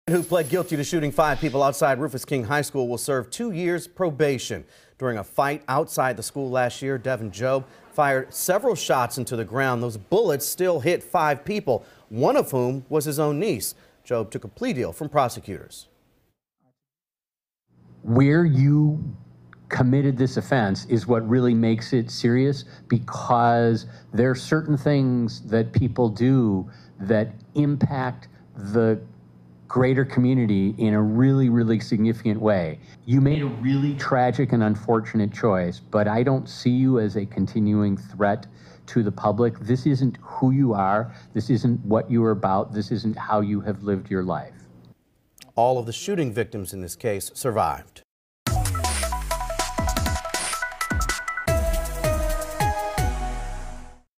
Man who shot five people outside Rufus King High School sentenced
During a fight outside the school last year, Devon Jobe fired several shots into the ground.